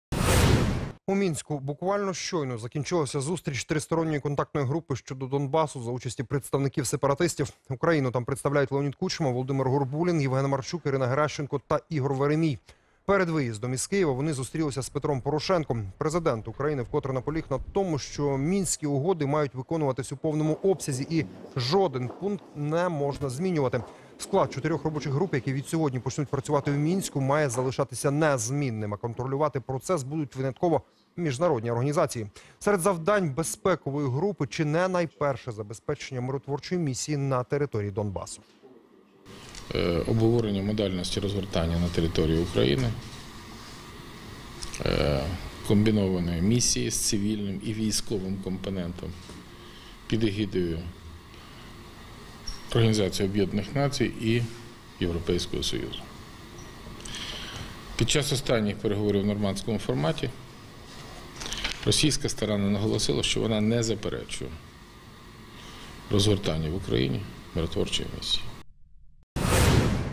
У Мінську закінчилася зустріч Тристоронньої контактної групи щодо Донбасу
Україну там представляють Леонід Кучма, Володимир Горбулін, Євген Марчук, Ірина Геращенко та Ігор Веремій.